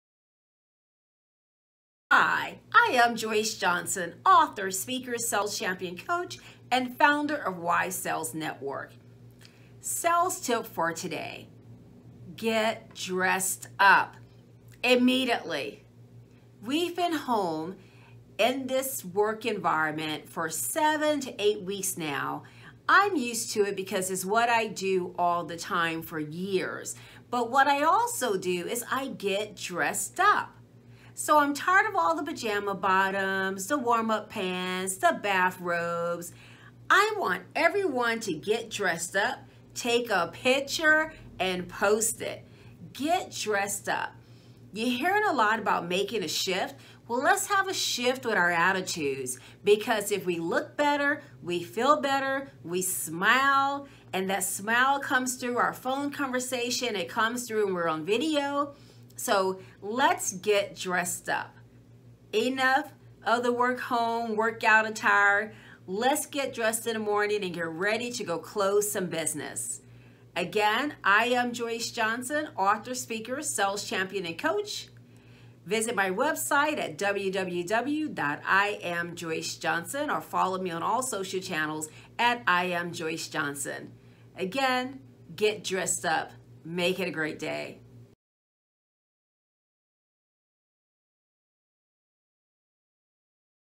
Ditch The Pajama Bottoms and Bath Robes | Lets Get Dressed Up & Close Some Business | Joyce Johnson
Joyce Johnson is an Author, Speaker, Coach, Sales Champion and Founder of "Why Sales Network" Today she offers up timely tips and advice for sales newbies and seasoned pro's! Today's sales tip: "Get Dressed Up"

Joyce Johnson
Author, Speaker, Sales Coach